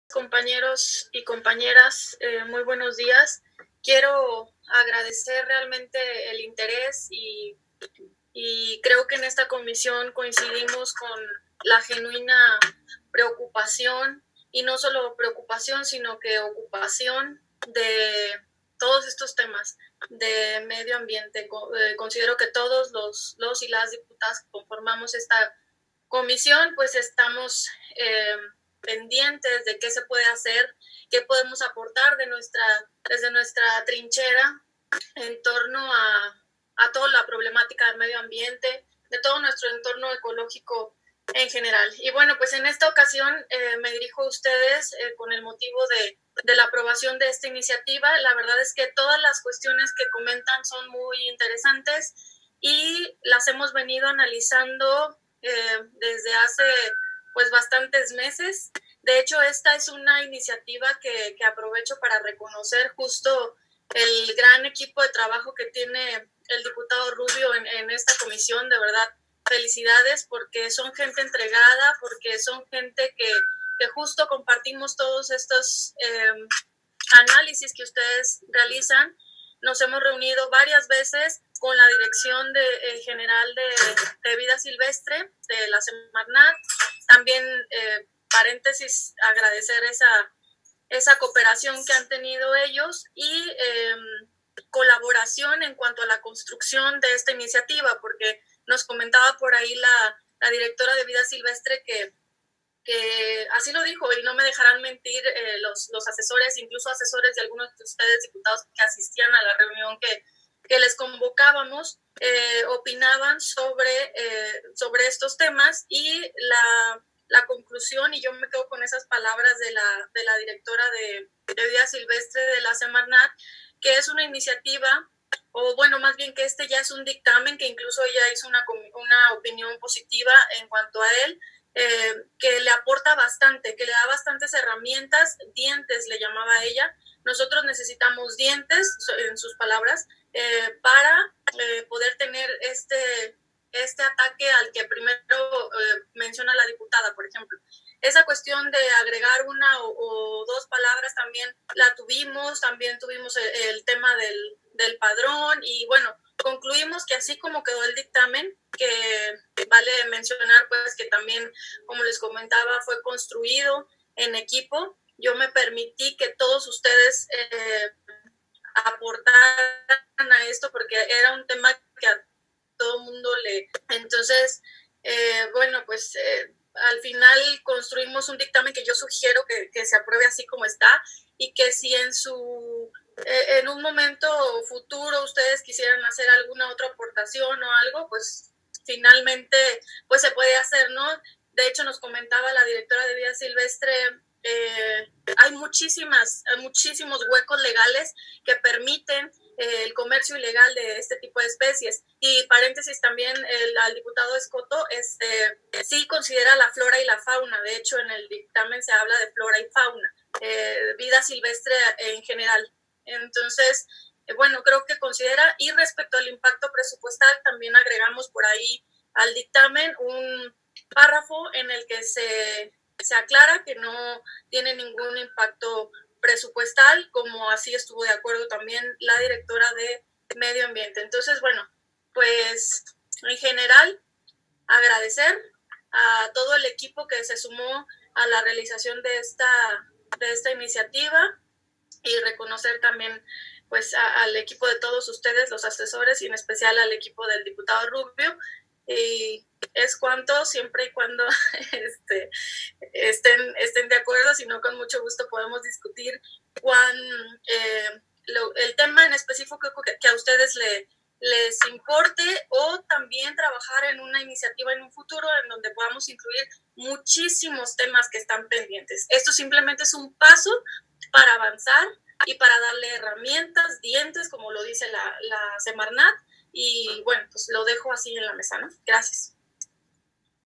31/07/2020 Participación de la Dip. Rosa María Bayardo Cabrera
Participación de la Dip. Rosa María Bayardo Cabrera en Reunión Virtual de la Comisión de Medio Ambiente, Sustentabilidad, Cambio Climático y Recursos Naturales